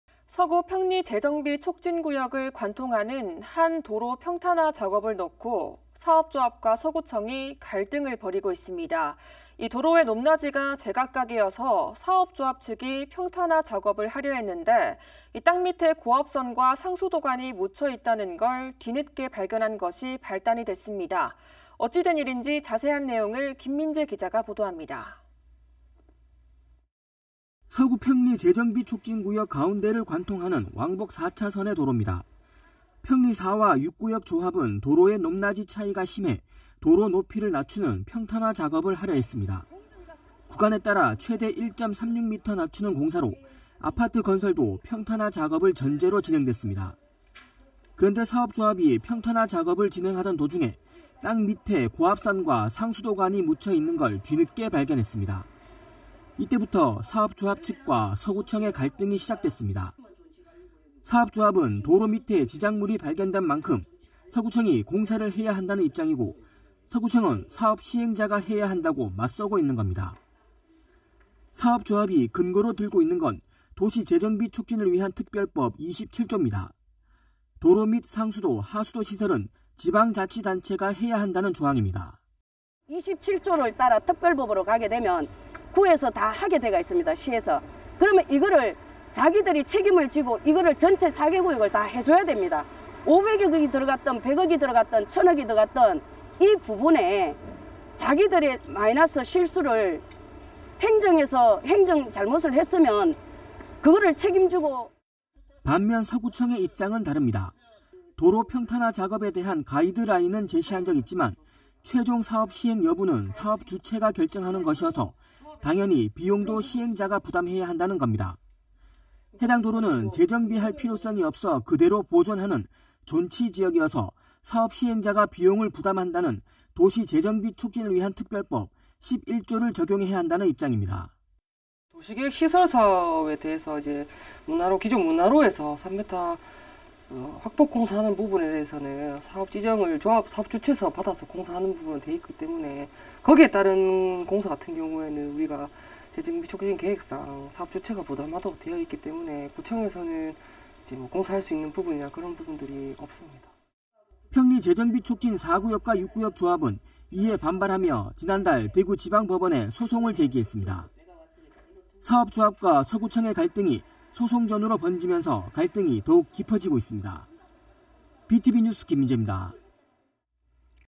[B tv 대구뉴스]도로 평탄화 공사 주체 놓고 책임 공방…소송전 이어져
서구 평리재정비촉진구역을 관통하는 한 도로 평탄화 작업을 놓고 사업조합과 서구청이 갈등을 벌이고 있습니다.